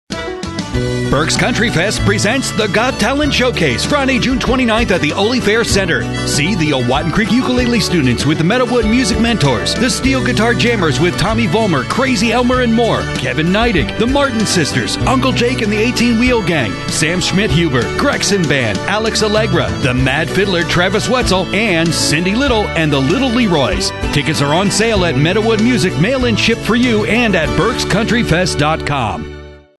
Got Talent Showcase video ad 30 second Berks Country Fest 2018
30 second promo ad for video, social media & TV

Pretzel City Productions, LLC

Produced by Dave Kline